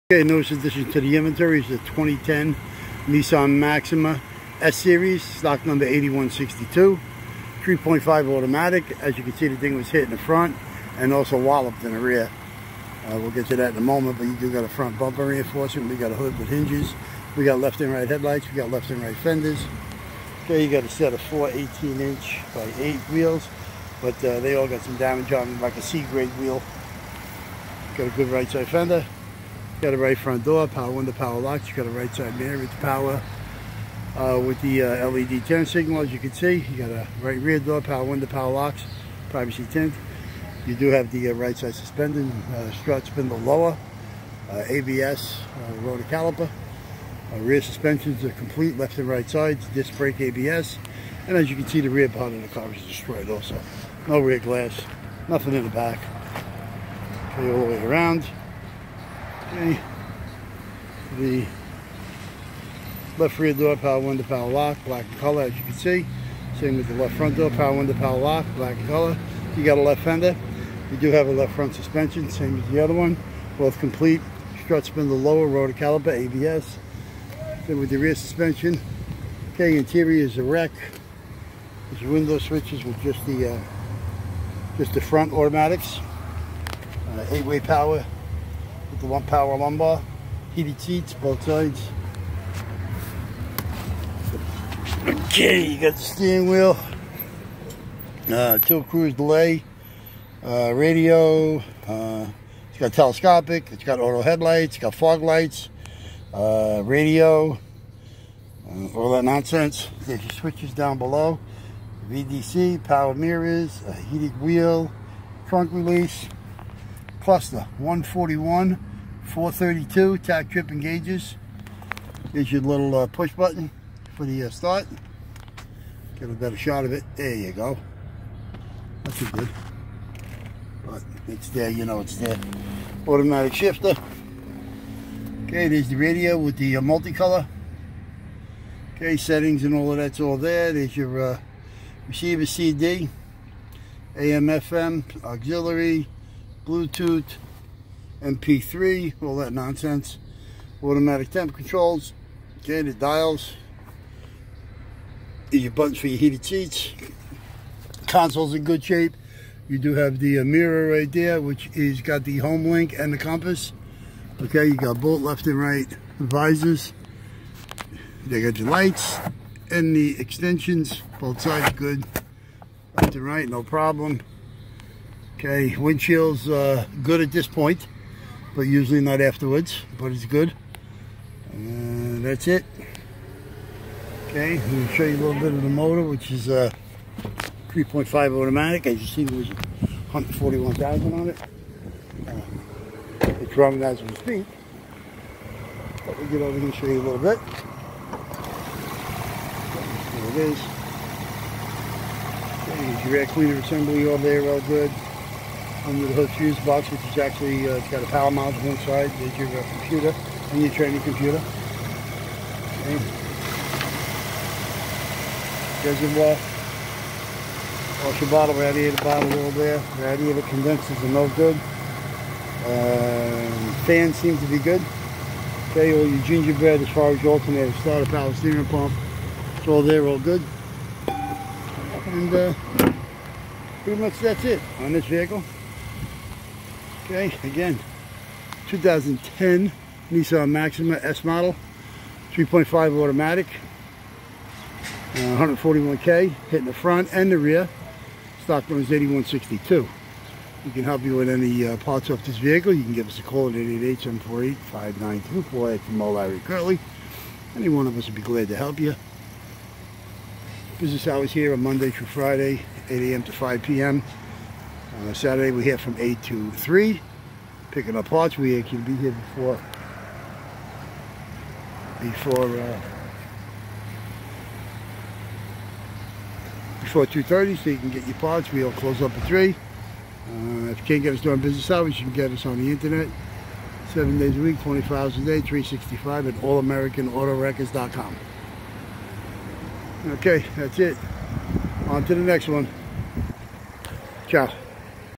All American Auto Wreckers is Dismantling this 2010 Nissan Maxima 3.5 Auto hit FRT & Rear Stock 8162
This 2010 Maxima is hit hard in the rear and also has front damage. It runs and drives good. Has 141k miles and sound good. This Maxima does have some good body parts for sale. The suspensions are complete front and back with abs brake system. This Maxima came with 18 inch alloy wheels and tires.. The 3.5 motor runs real nice and is ready for sale. We sold the transmission so that is gone already. This 2010 Maxima is a S Model with some nice options. The exterior is Paint code-KH3-Black and the Interior trim Code is -G Black. The Build date is 3-8-09. So if Moe-Larry or Curly can help you with any parts from this 2010 Nissan Maxima PLEASE Call Us Toll-Free at or go